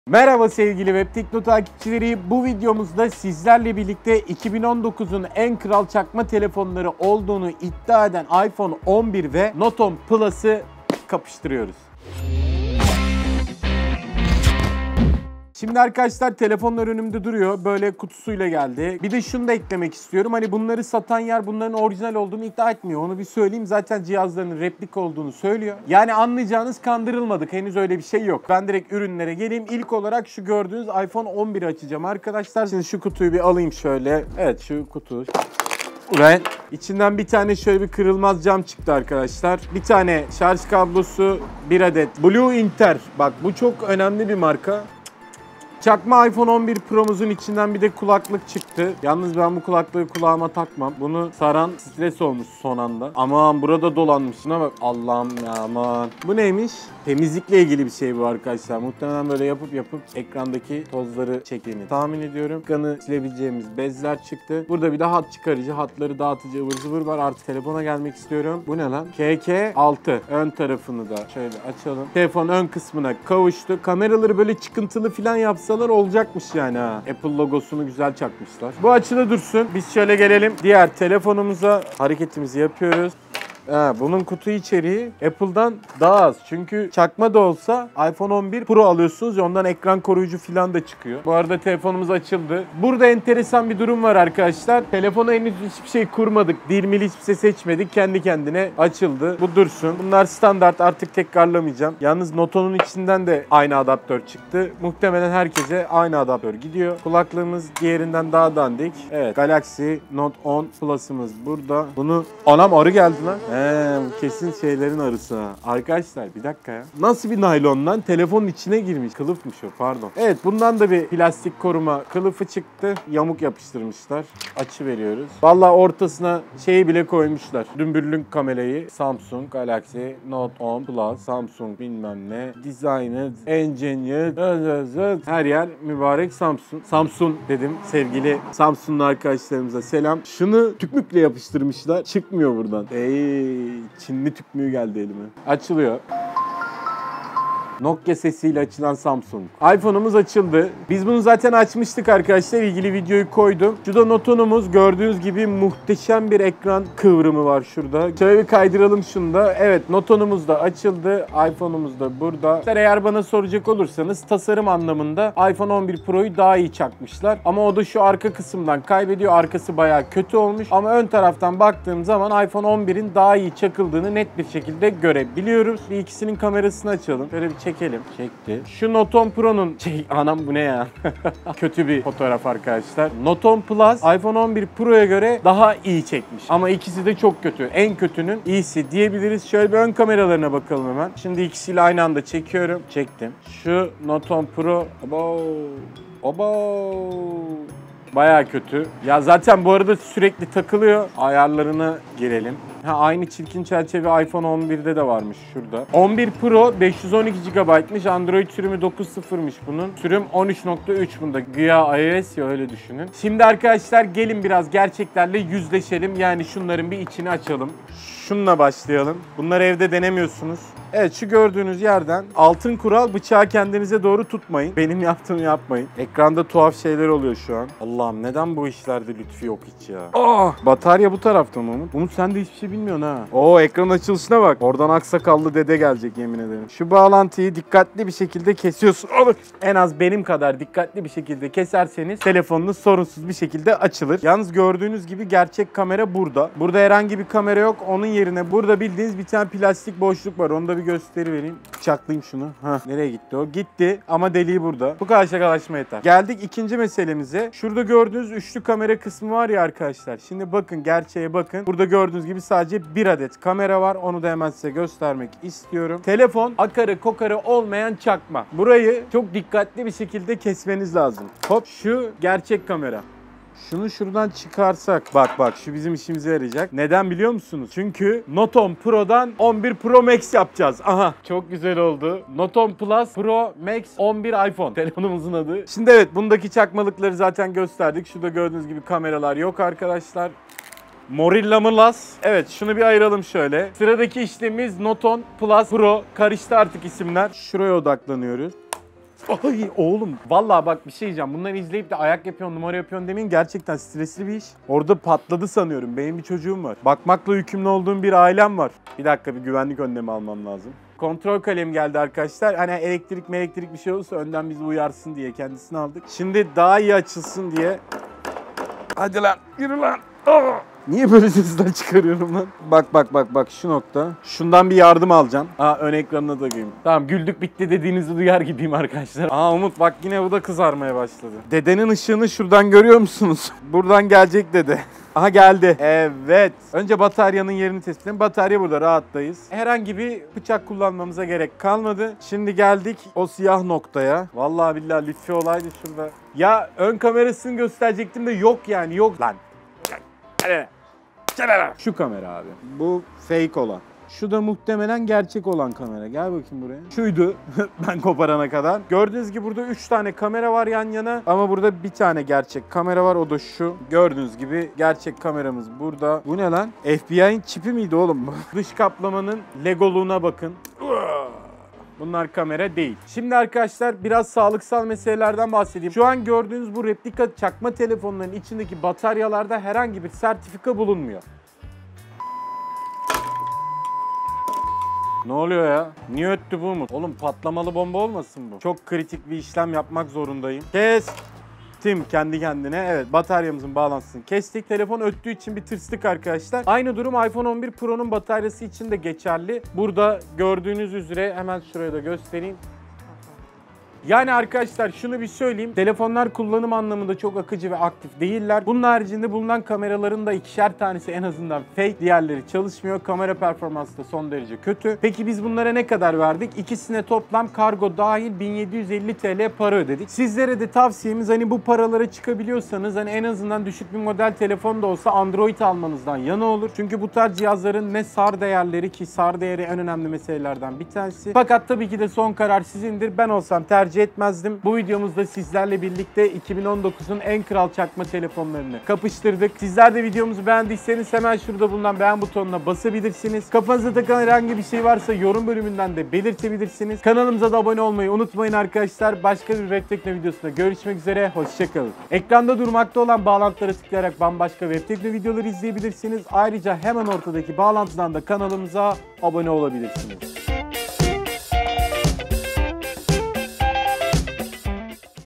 2019'un En Kral Çakma Telefonları: Çakma iPhone 11 Pro ve Note 10+ Aynı Videoda Çaktık!
2019'un En Kral Çakma Telefonları: Çakma iPhone 11 Pro ve Note 10+ Aynı Videoda Çaktık!

Merhaba sevgili Webtekno takipçileri! Bu videomuzda sizlerle birlikte 2019 yılının en sağlam çakma telefonları olan iPhone 11 Pro Max ve Note 10+'ı kapıştıracağız.

Keyifli izlemeler :)